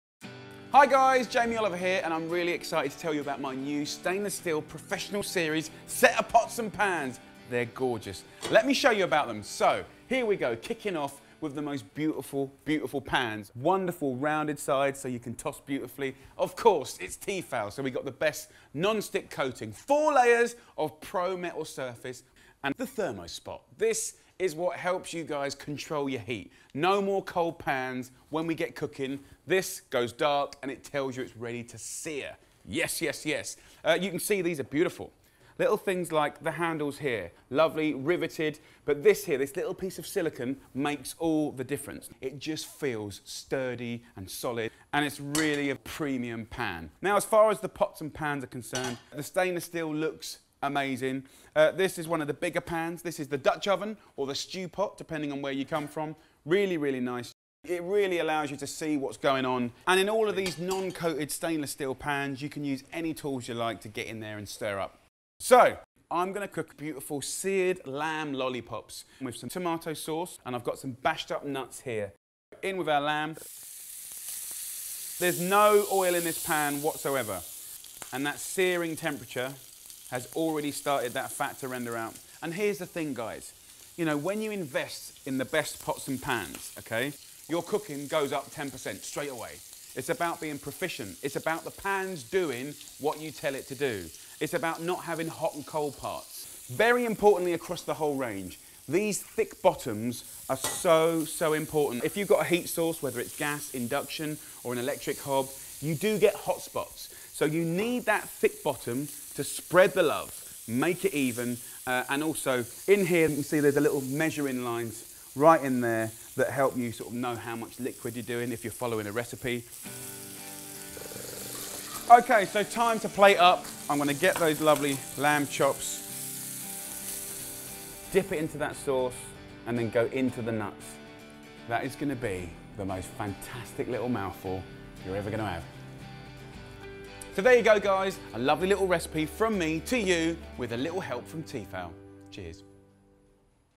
Tefal Jamie Oliver Professional Stainless Steel
Tefal has developed a great new cookware range with Jamie Oliver. Jamie Oliver Professional Stainless steel is made of long-lasting stainless steel which has great heat retention. The pans are of course with Tefal's unique temperature indicator THERMO-SPOT® as well as the great non-stick coating which allows you too cook without any oil.